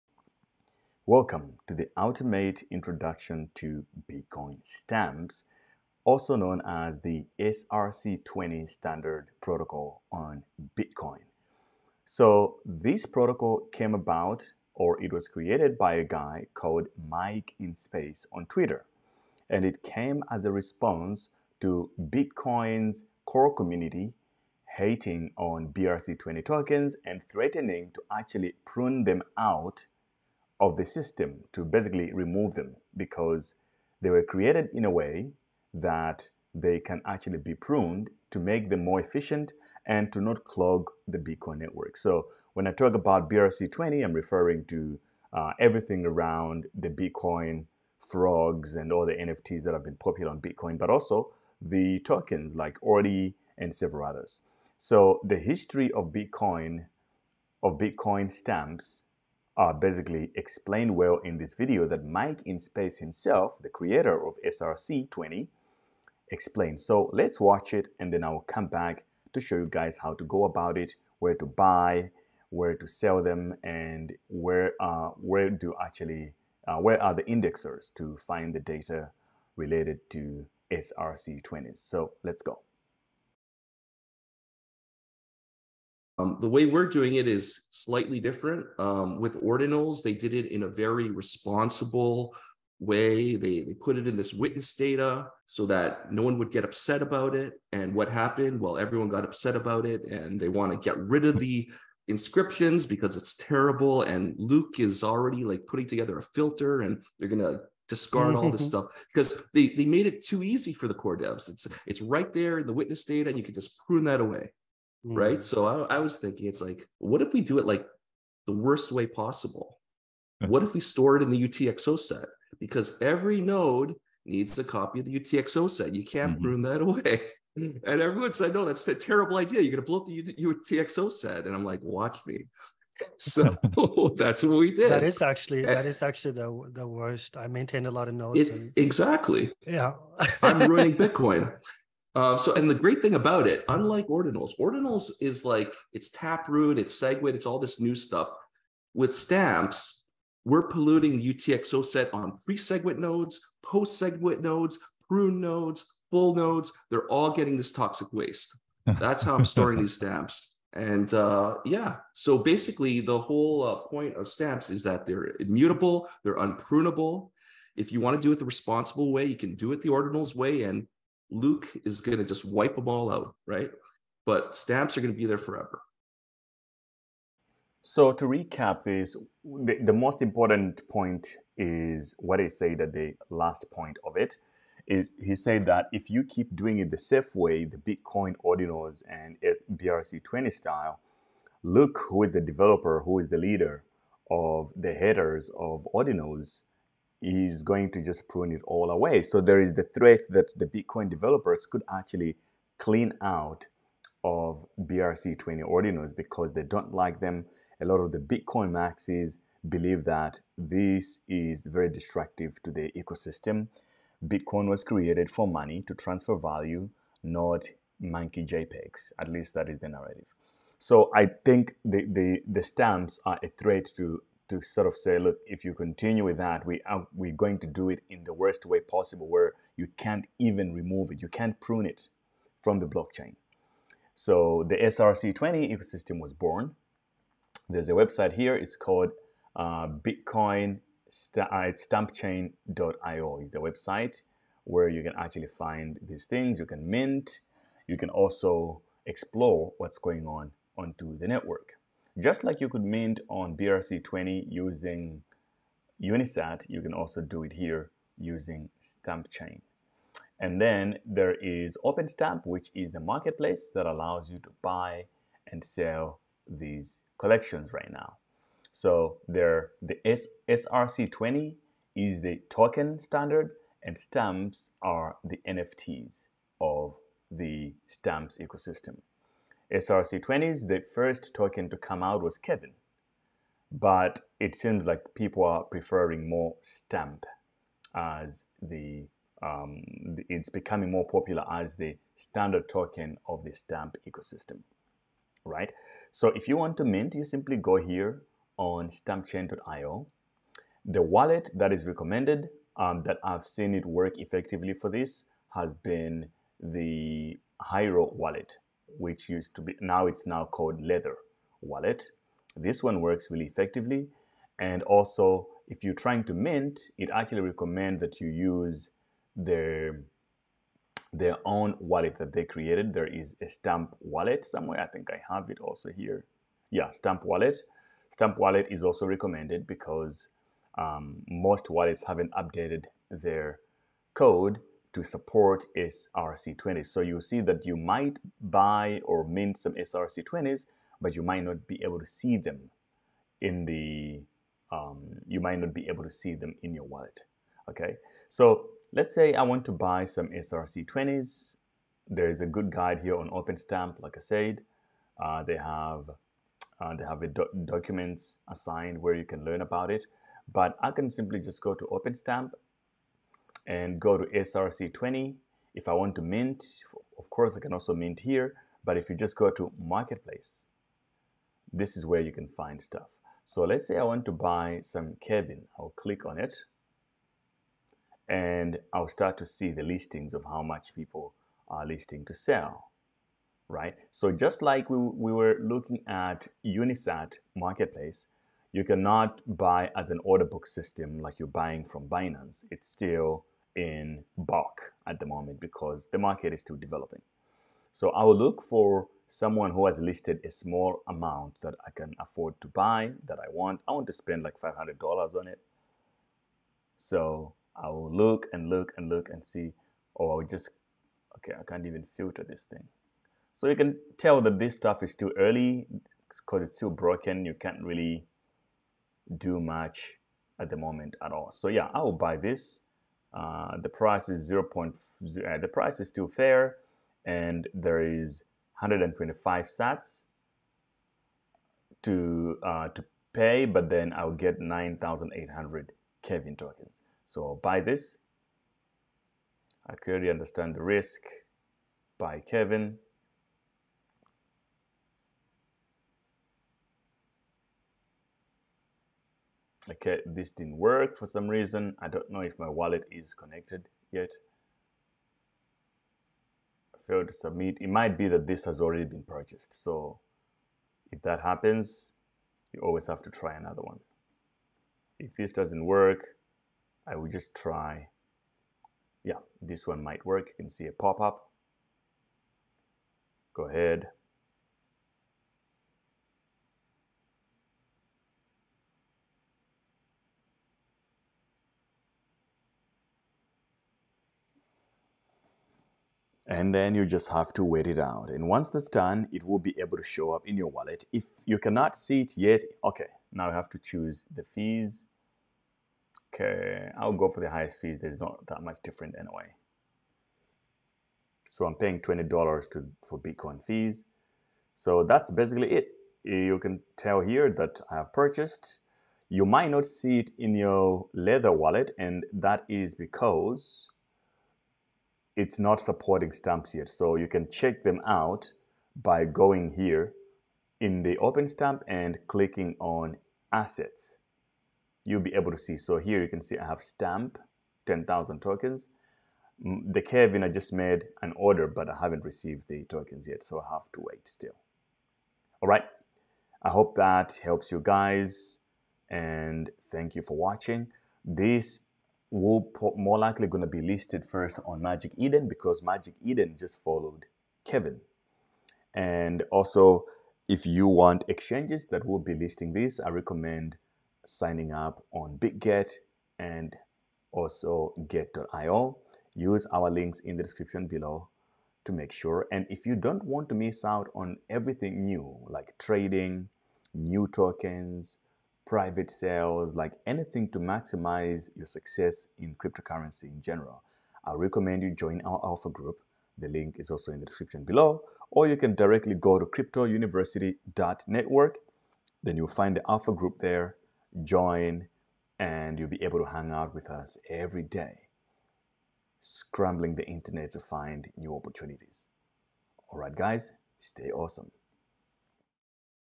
The Ultimate Guide to Bitcoin Stamps SRC20 Tokens
This is an introduction to a series of videos focused on the new standard in Bitcoin called; SRC20 TOKENs Ordinals. You will learn how to mint SRC20 tokens using Cyborg Crafter and Unisat Wallet.